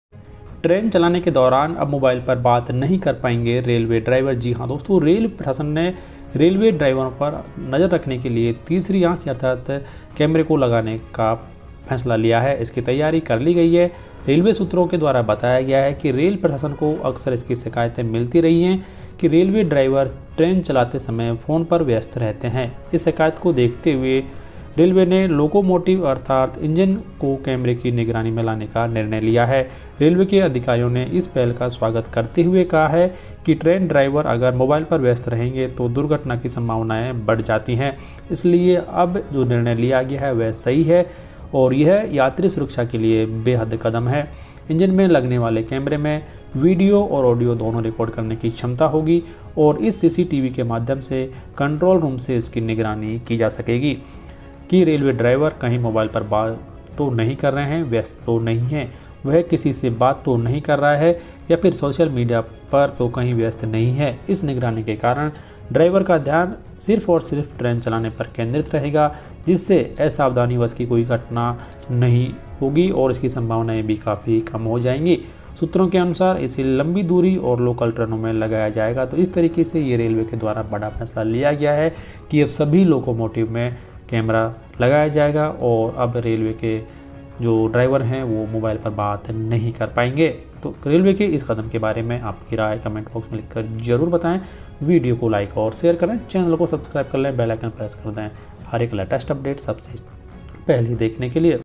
Duty पर नहीं होगा Mobile इस्तेमाल। Latest Order| Govt Employees News
Today Latest News: ड्यूटी पर मोबाइल उपयोग पर लगी रोक। Govt Employees News. Railway employees news Today. Mobile use ban while driving.